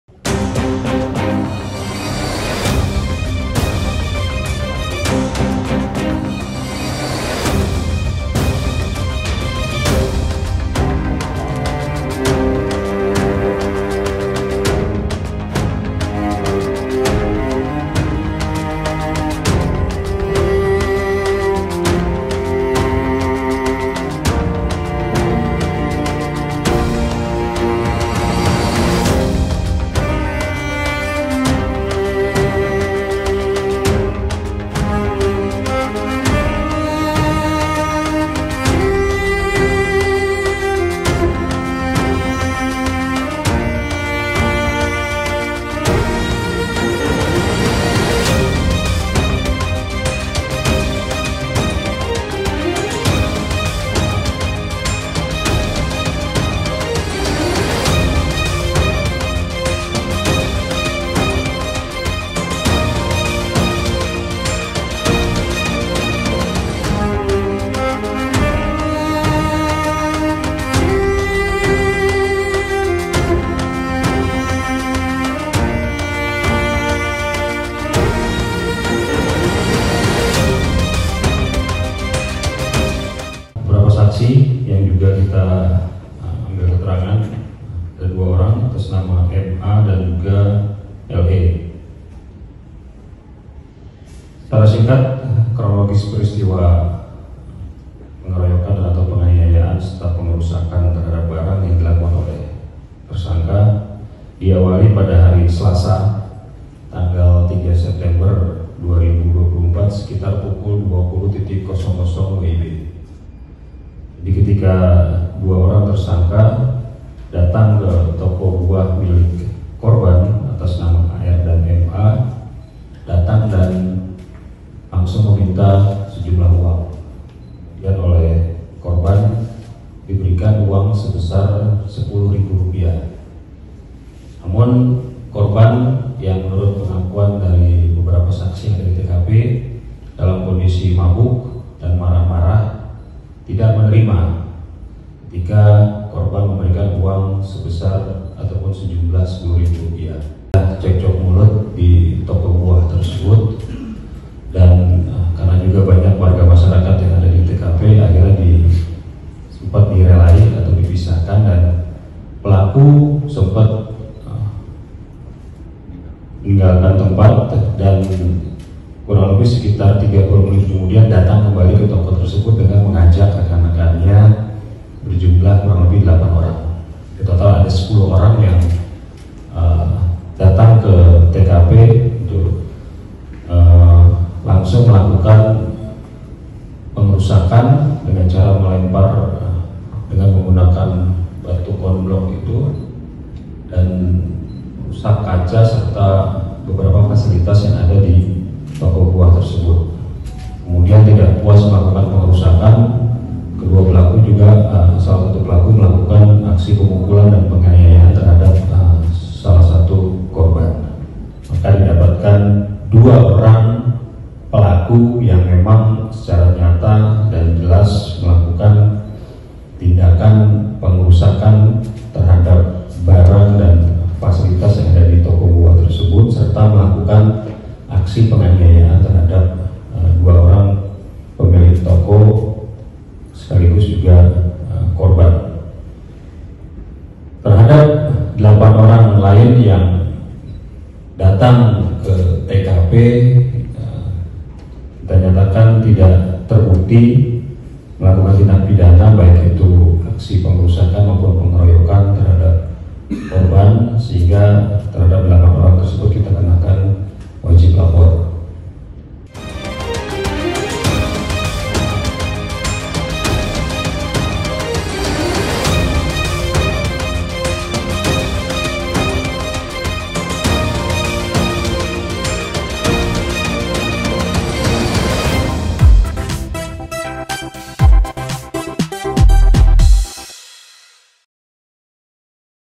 Polisi Ungkap Kronologi Oknum Ormas Keroyok Pedagang Buah di Jakarta Barat
Polisi menetapkan 2 orang oknum organisasi masyarakat (ormas) yang melakukan pengeroyokan kepada pedagang buah di Jalan Joglo Raya, Kembangan Jakarta Barat, sebagai tersangka.

Keduanya mengeroyok korban karena tak terima hanya diberi Rp10 ribu. Pelaku yang menurut pengakuan beberapa saksi di TKP dalam kondisi mabuk dan marah-marah.

Kini, polisi hanya menetapkan dua orang tersebut sebagai tersangka. Untuk delapan lainnya tidak terbukti melakukan tindak pidana, sehingga hanya dikenakan wajib lapor.